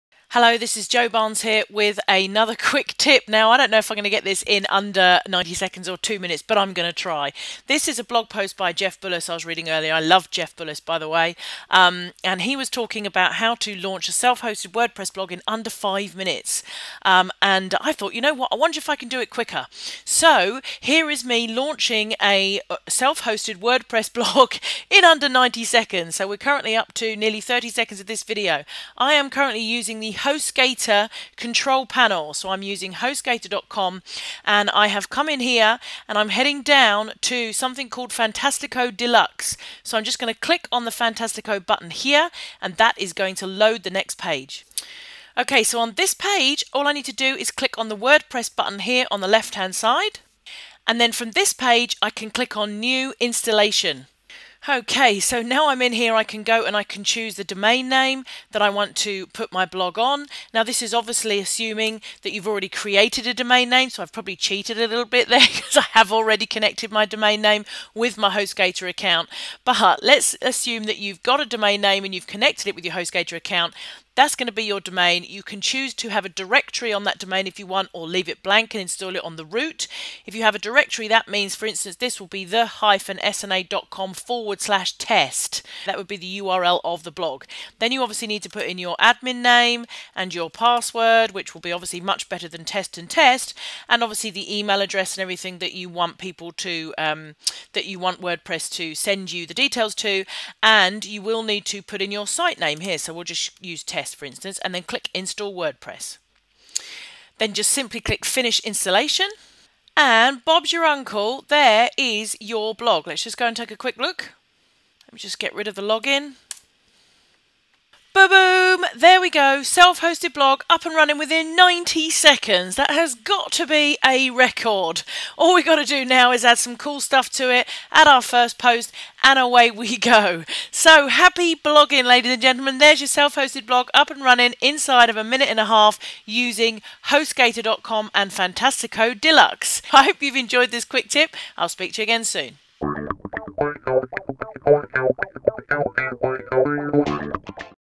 How to Install a Self Hosted Wordpress Blog in Under 90 Seconds
A quick tutorial using HostGator and Fantastico Deluxe to install a Self-Hosted Blog in Wordpress in 90 seconds. Then fully connecting the accounts and testing them.
This means you can integrate the two and feed more fresh content to your website which is great for SEO.
It's always good to 'test' this first but this is incredibly straight forward and will save you time and unnecessary navigation.